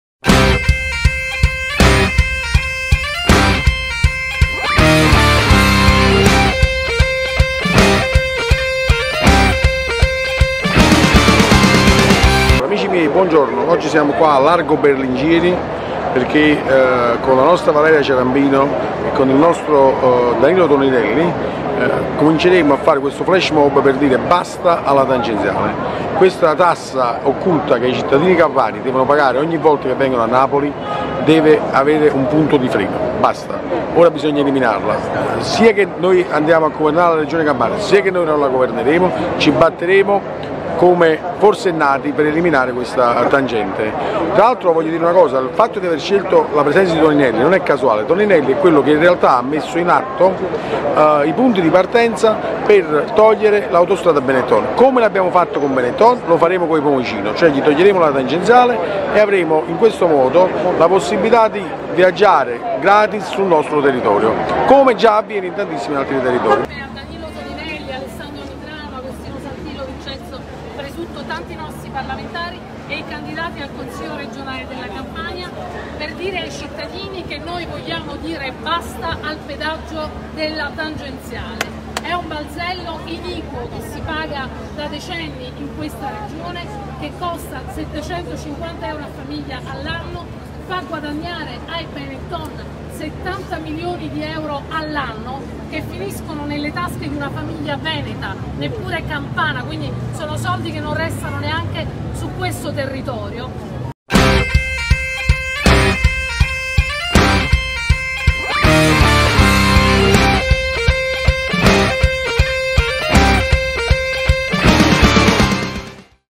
Stop al pedaggio in tangenziale di Napoli
Con il Movimento Cinque Stelle elimineremo il pedaggio in Tangenziale di Napoli